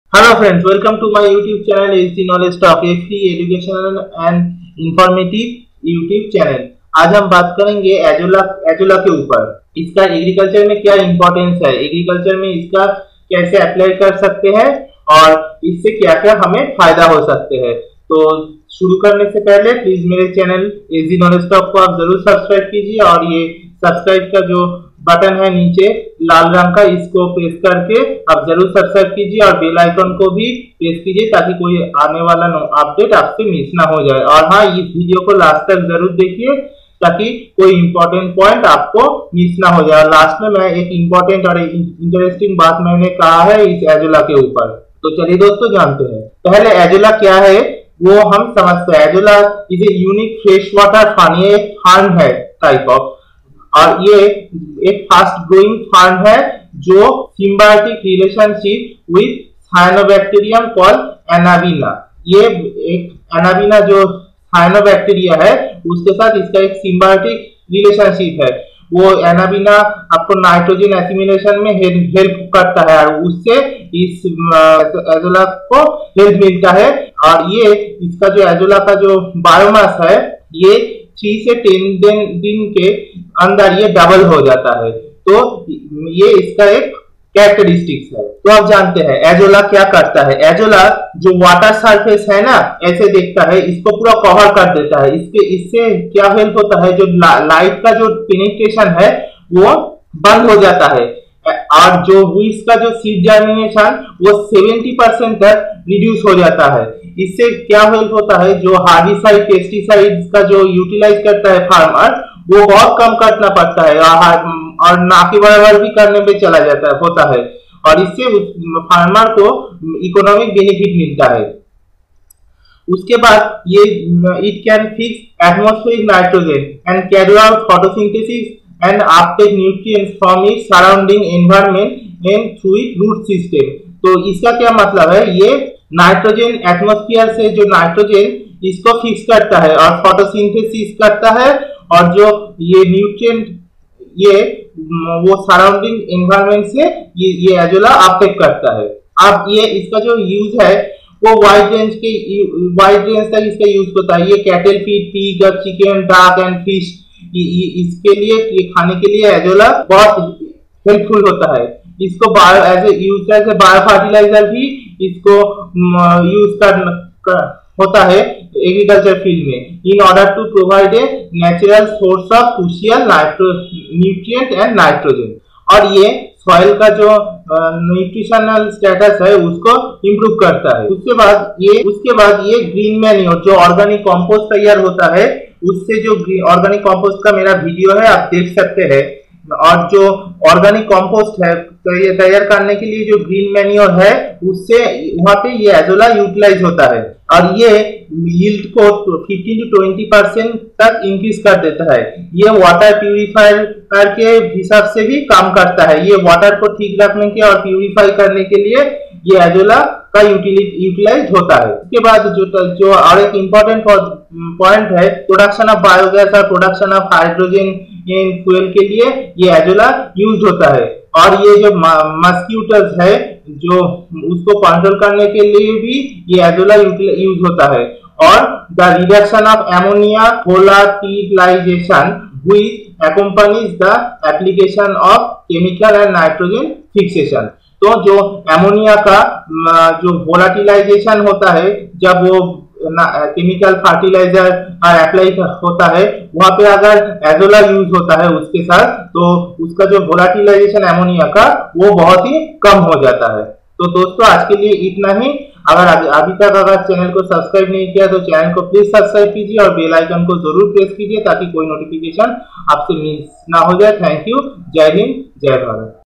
Azolla-Important role in Agriculture
Importance of Azolla in  Agriculture.
Azolla benefits :
Organic Approach
Environmental ,Economic ,Crop yielding ,Bio-fertiliser  ,Bio-gas production ,Water purifier Nitrogen Assimilation etc.